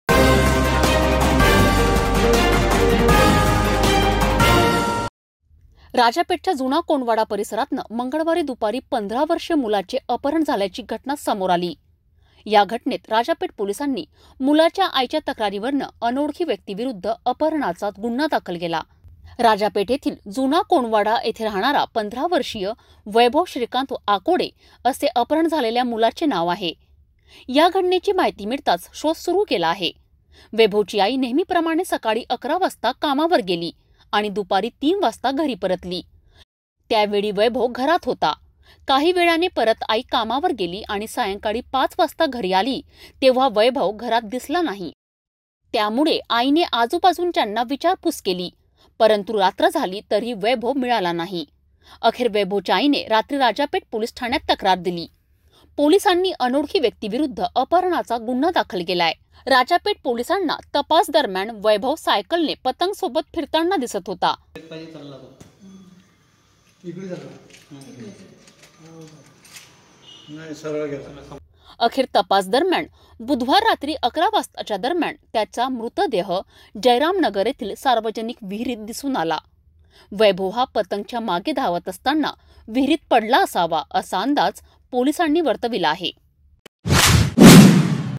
१५ वर्षीय वैभव विहिरीत मृतअवस्थेत सापडला
राजापेठ पोलिसात केली होती तक्रार पतंगचा नादात जिव गेला असा पोलिसांचा अंदाज